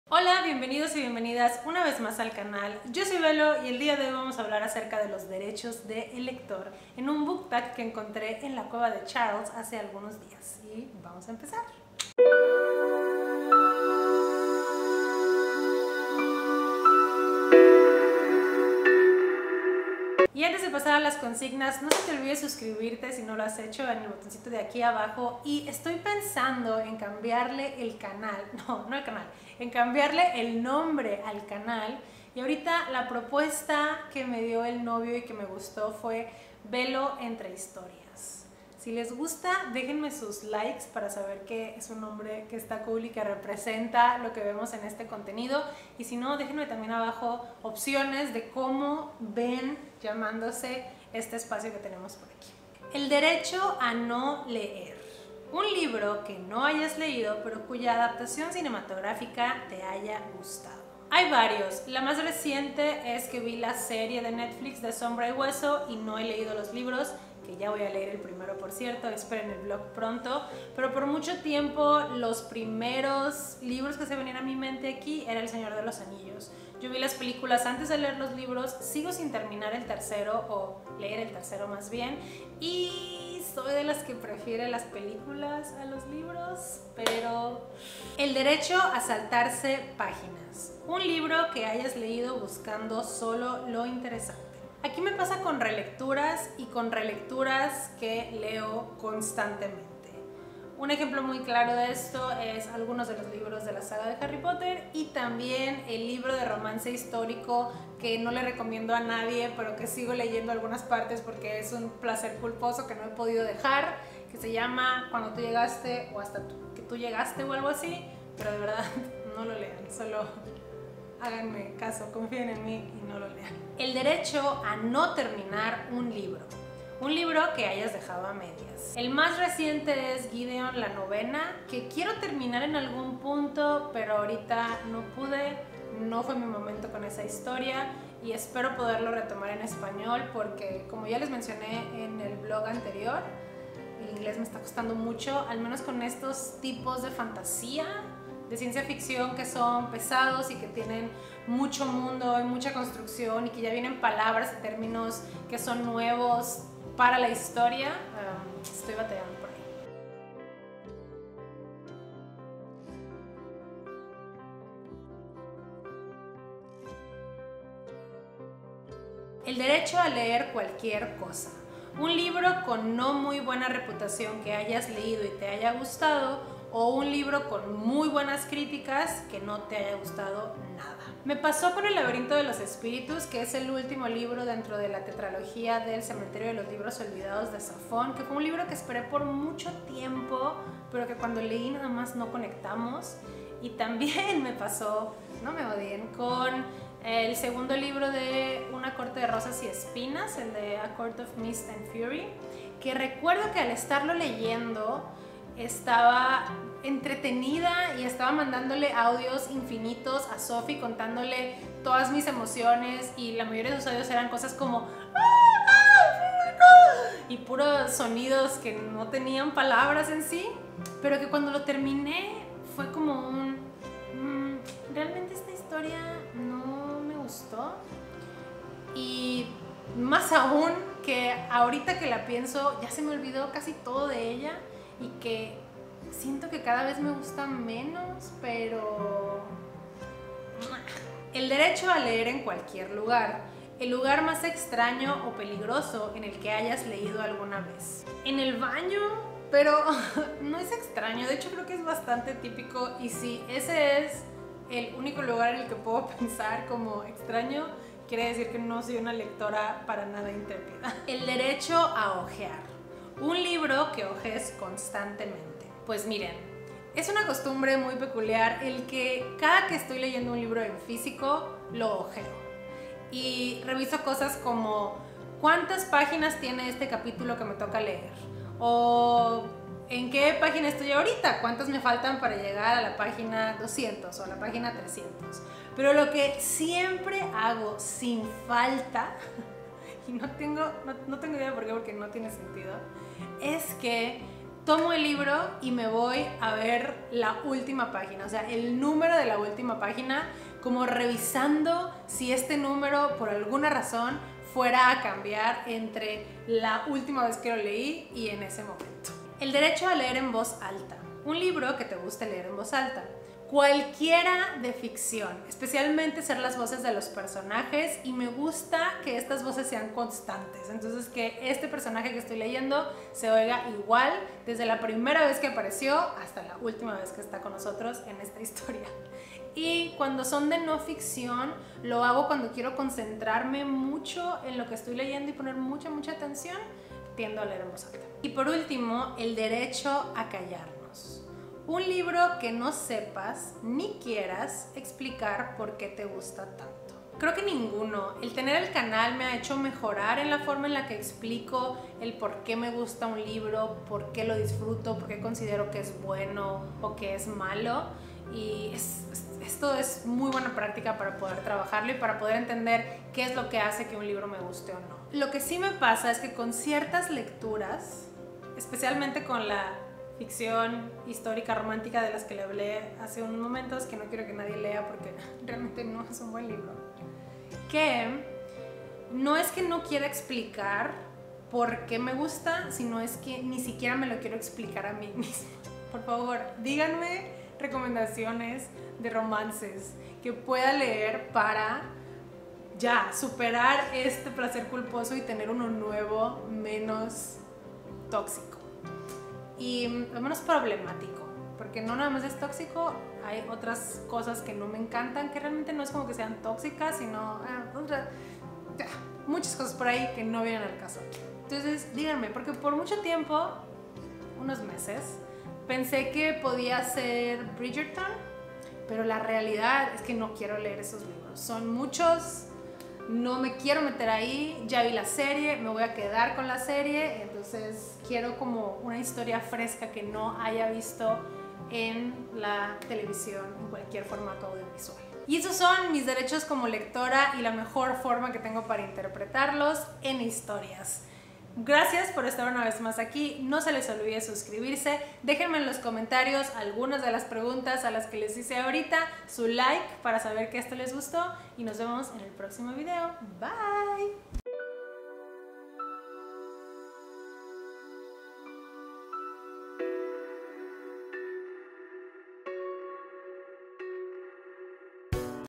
BOOKTAG LOS DERECHOS DEL LECTOR// ¿no leer? ¿dejar libros a medias? ¿preferir la película?
Hoy tenemos el booktag de los derechos del lector que vi en el canal de la cueva de Charles hace algunos días.

Consignas:
1. EL DERECHO A NO LEER. Un libro que no hayas leído pero cuya adaptación cinematográfica te haya gustado.
2. EL DERECHO A SALTARSE PÁGINAS. Un libro que hayas leído buscando solo lo interesante.
3. EL DERECHO A NO TERMINAR UN LIBRO. Un libro que hayas dejado a medias.
4. EL DERECHO A RELEER. Un libro que hayas leído varias veces.
5. EL DERECHO A LEER CUALQUIER COSA. Un libro con no muy buena reputación que hayas leído y te haya gustado, o un libro con muy buenas críticas y no te haya gustado nada.
6. EL DERECHO A LEER EN CUALQUIER LUGAR. El lugar más extraño o peligroso en el que hayas leído alguna vez.
7. EL DERECHO A HOJEAR. Un libro que hojees constantemente.
8. EL DERECHO A LEER EN VOZ ALTA. Un libro que te guste leer en voz alta.
9. EL DERECHO A CALLARNOS. Un libro que no sepas (ni quieras) explicar por qué te gusta tanto.

¿QUIERES 2 MESES GRATIS EN SCRIBD?